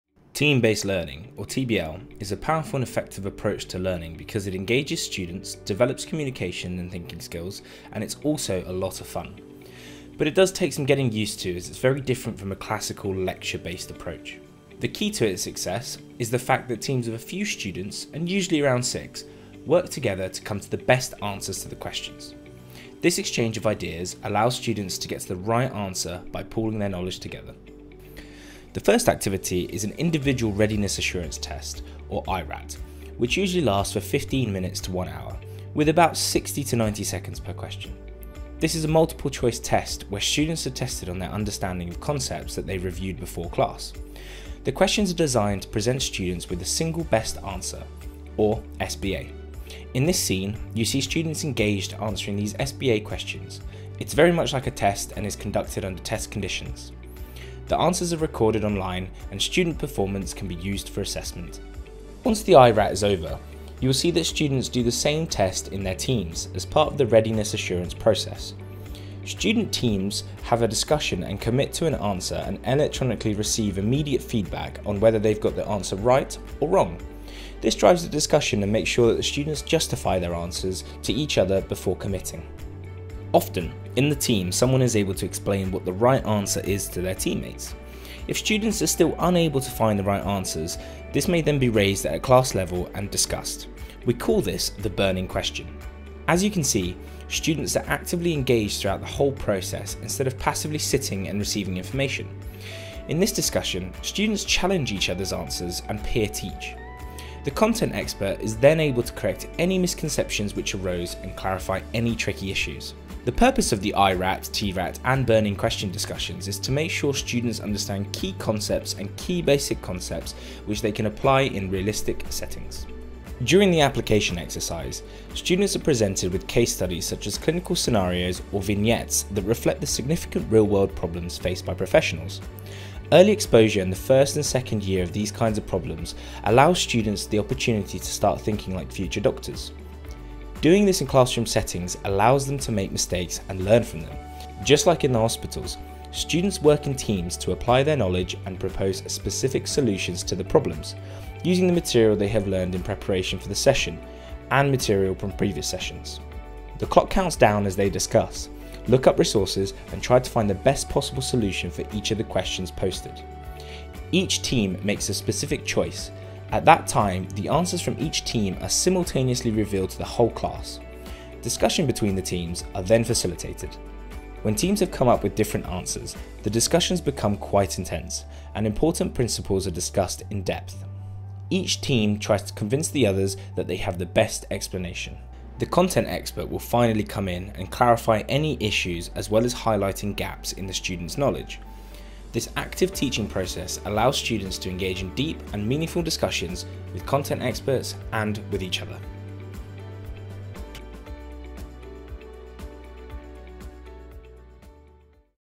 Team Based Learning (TBL) at Brunel Medical School, Brunel University London
Discover everything there is to know about Team-Based Learning (TBL) at Brunel Medical School, Brunel University London.

Brunel Medical School uses TBL as the main method of classroom teaching instead of lectures. Working in teams of six in a purpose-designed TBL classroom, students exchange ideas and refine their understanding of the topics through discussions.

Students solve problems and debate whilst developing life-long, transferable skills - highly relevant to medicine, under the guidance of clinicians and scientists.